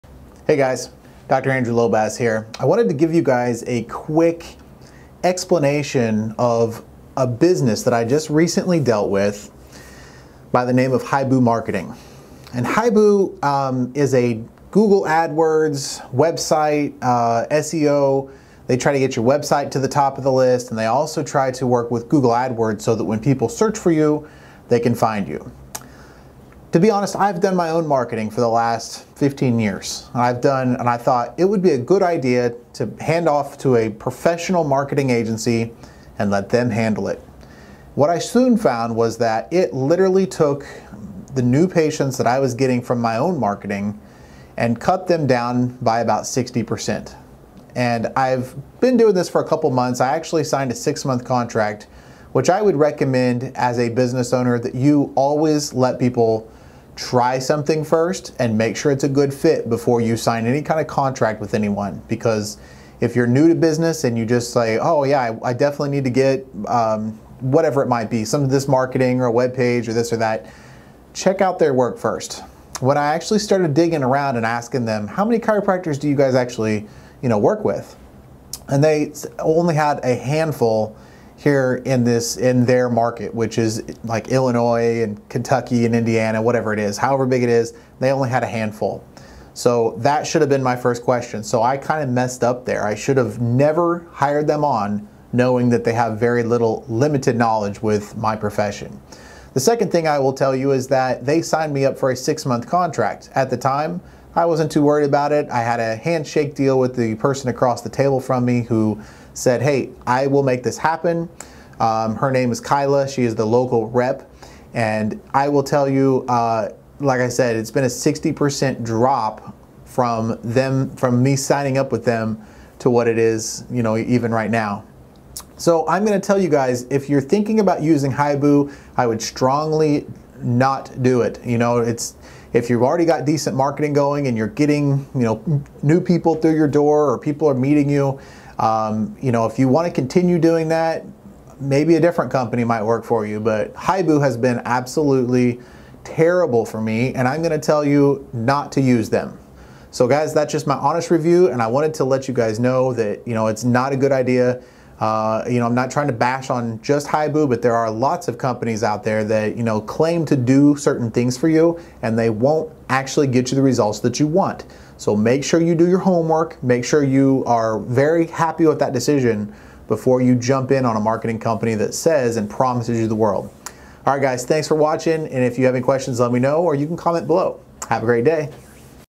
Is Hibu marketing good?
I’ve done my own Google AdWords for years, but I was told that they would far surpass my efforts and I would get more bang for my buck as they had special connections with google.

If your new to Google AdWords, I suggest you take a course and learn how to do it yourself.   Paying a company to run ads for you is mainly filling their pockets with their fees and some of the budget spend on actually getting you clients.

Hibu dm has done me a huge dis-service. They took my office down 60%, sometimes lower and they just keep saying, give it time it has to ramp up. This has been a really long two months since they started. of ramp up time.

This is only my opinion, and maybe it has worked for some people.

I personally can’t wait till my contract is over so I can continue using AdWords to grow my practice.

Really consider if it’s worth the headache.   Check out the bbb on them.   It’s not pretty, and now I see why.